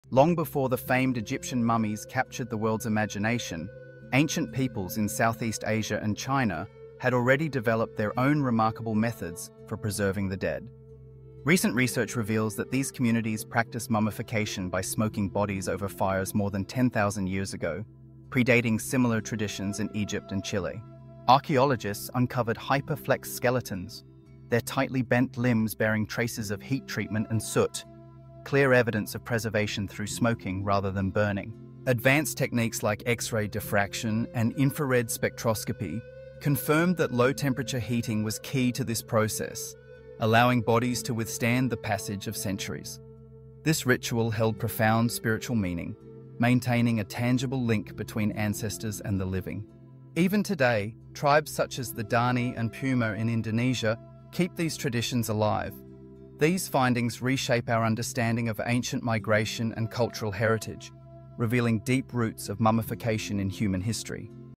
Ancient Smoking Mummies - The world's oldest mummies are not Egyptian
🕯️ The world's oldest mummies are not Egyptian
A new study shows that mummification by smoking bodies was practiced in Southeast Asia and China more than 10,000 years ago, earlier than in Egypt or Chile.
Archaeologists found hyperflexed skeletons (bodies in a tightly bent position) with signs of heat treatment and soot, indicating smoking over a fire.
🔬 How it was studied
X-ray diffraction and infrared spectroscopy were used to confirm the effects of low-temperature heating.
It was found that the bodies were not burned, but smoked, which allowed them to be preserved for tens or hundreds of years.
🌏 Cultural significance
Smoking had spiritual and ritual significance, allowing the presence of ancestors to be preserved among the living.
Modern tribes, such as the Dani and Puma in Indonesia, still practice similar rituals.
🧬 Migration and Heredity
The findings support the theory of a “two-layer” migration: hunter-gatherers arrived in the region before farmers.
The practice of mummification could be a cultural legacy of the first Homo sapiens who came from Africa.
This discovery changes our understanding of where and how the practice of mummification originated. If you want, I can tell you more about modern burial rituals in different cultures or how mummies are stored in museums.